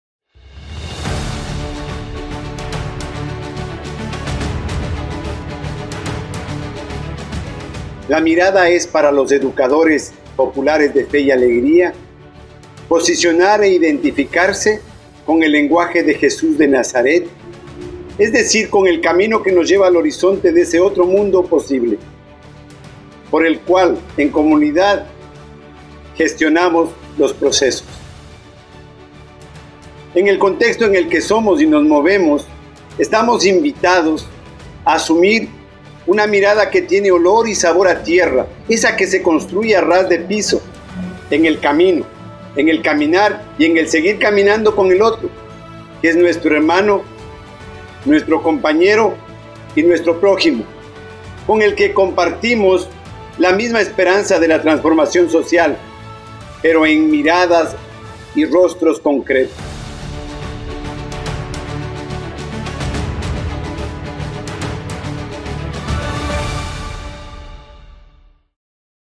Estamos invitados a caminar con el otro, nuestro hermano.
"Educar es enseñar a mirar. Hoy, ¿quién es nuestro prójimo?", es el leitmotiv institucional que hemos preparado para este 2022. La intención es que nuestro quehacer educativo se fortalezca, desde la recreación cotidiana de nuestra misión, visión, valores institucionales y modos de proceder, que inciden en el compromiso ético-político de cada uno de nosotros/as; alimentando la "chispa" para "seguir  movilizándonos,  por una verdadera  innovación  educativa  que  implica  una transformación  social  desde  el derecho y la justicia educativa"